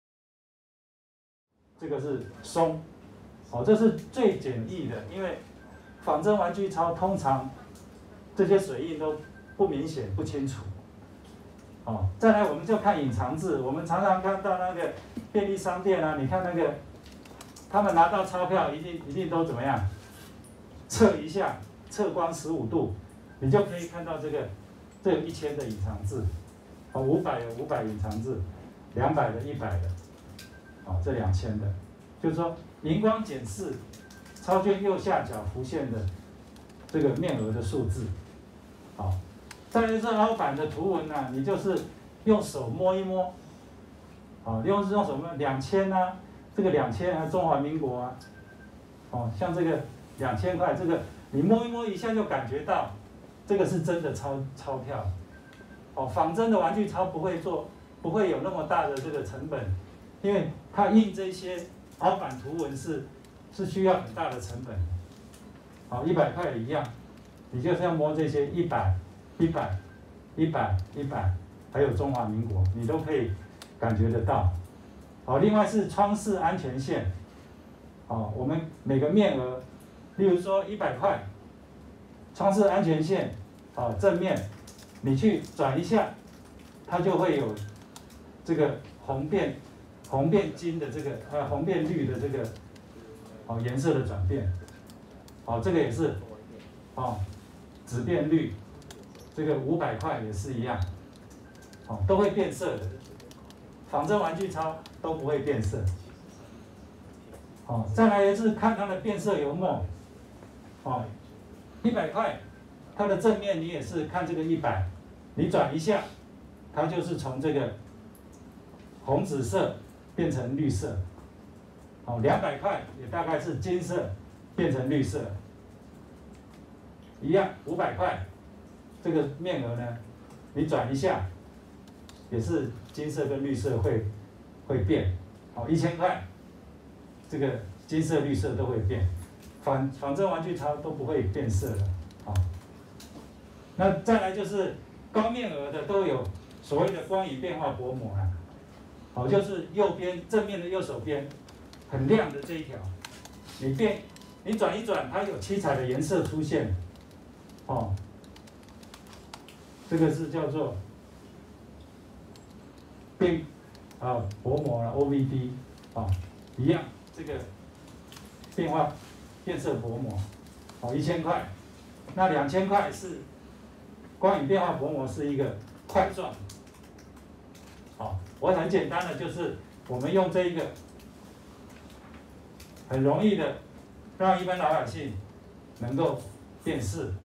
仿真玩具鈔詐騙 央行提醒分辨秘訣 |中央社即時影音
近期有心人士透過仿真玩具鈔進行詐騙，央行17日舉行記者會表示，這確實是新型態的詐騙手法，呼籲民眾拿到鈔票後多看幾眼，掌握3原則就能辨別真偽。（中央社記者王騰毅攝）